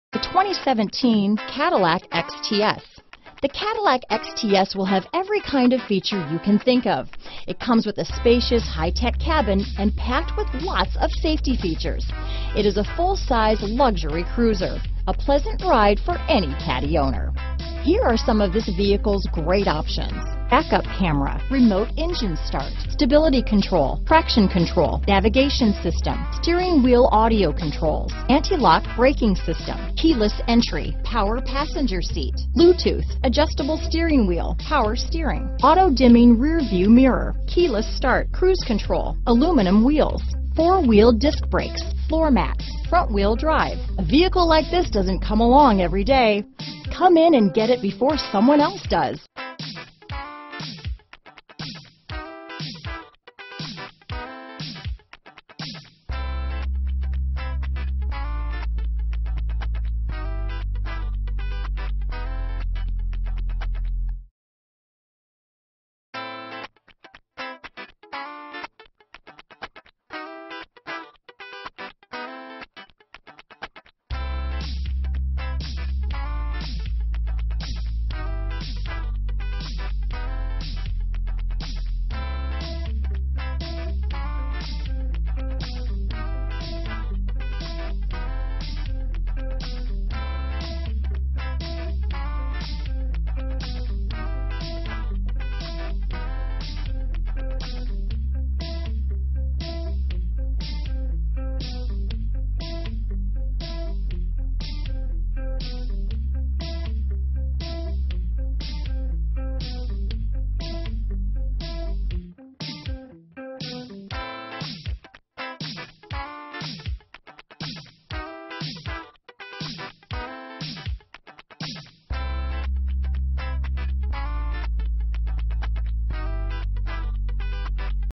2017 Cadillac XTS Orange County, Irvine, Laguna Niguel, Newport Beach, Mission Viejo, CA 58127
2017 Cadillac XTS

Family owned & operated in South Orange County for over 69 years.2017 Cadillac XTS LuxuryBlack Raven, Jet Black, Automatic Parking Assist, Dual-Outlet Stainless-Steel Exhaust, Front & Rear Park Assist, Leather-Wrapped Steering Wheel, Luxury Package 1SB, Navigation System, Upper & Lower Auxiliary Daytime Running Lamps.